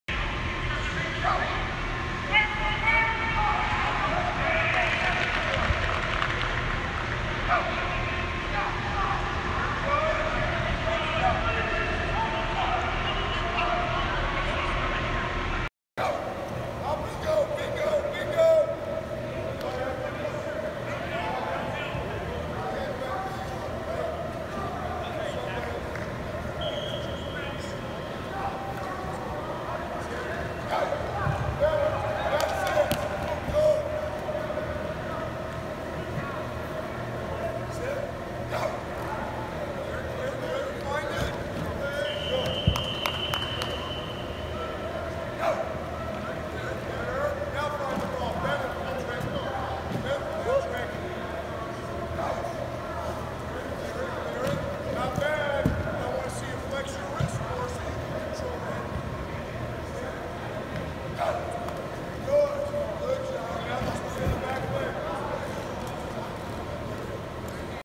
Pitt DL Cut Block Drill 9/11/18
Pitt defensive linemen, under the instruction of coach Charlie Partridge, practice avoiding cut blocks as Pitt prepares to play Georgia Tech on Saturday.